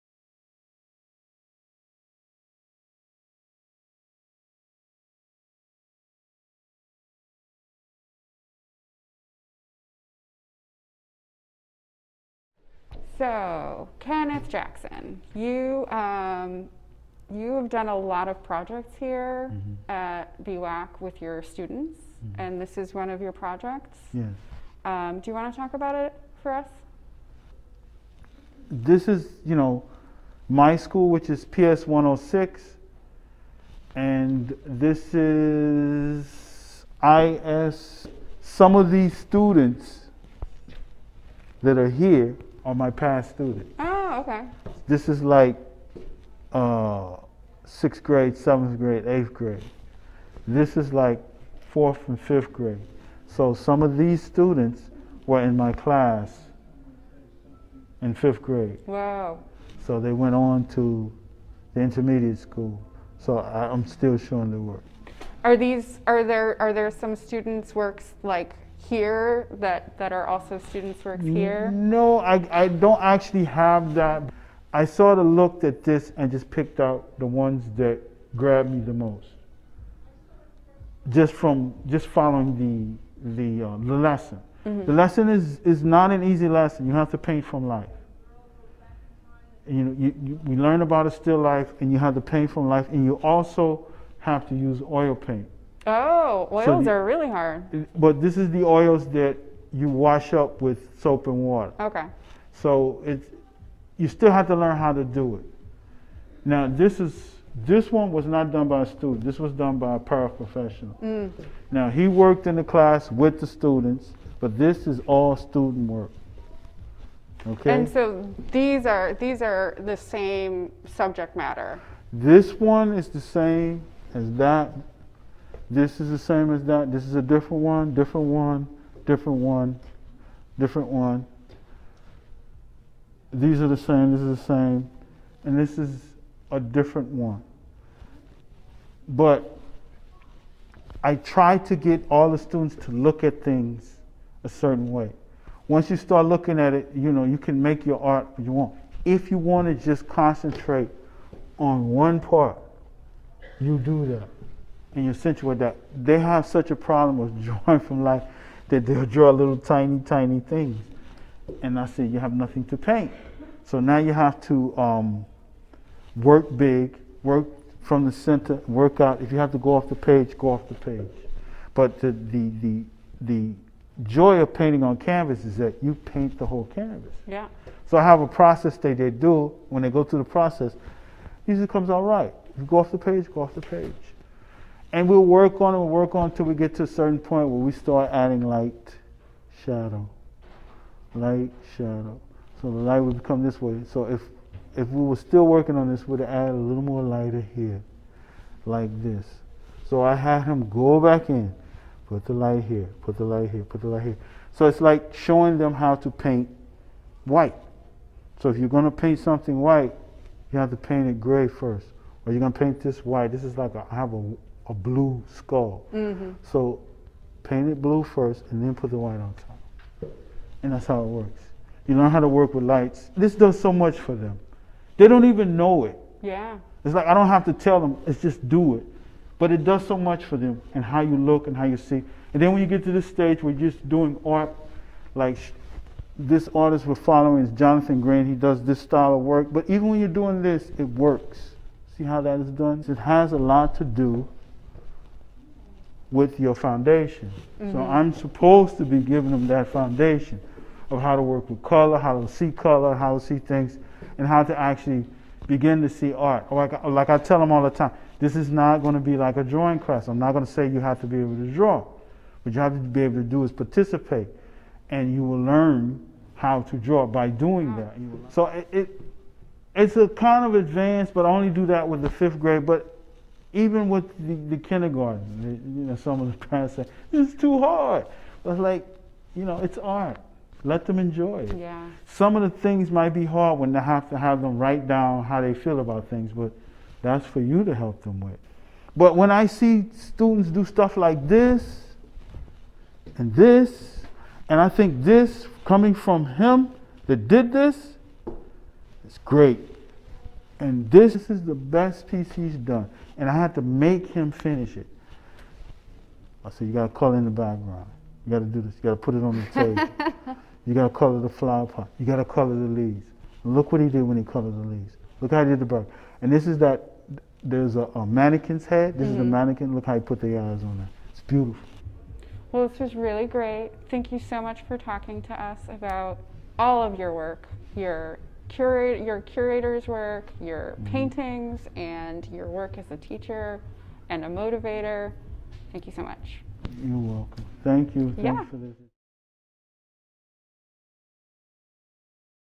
BWAC Kenneth Jackson Educator
BWAC artist Kasia Zurek-Doule speaks to BWAC curator, artist and educator, Kenneth Jackson about his students' works, on exhibition during the fall 2020 season. Interview dated: October 17, 2020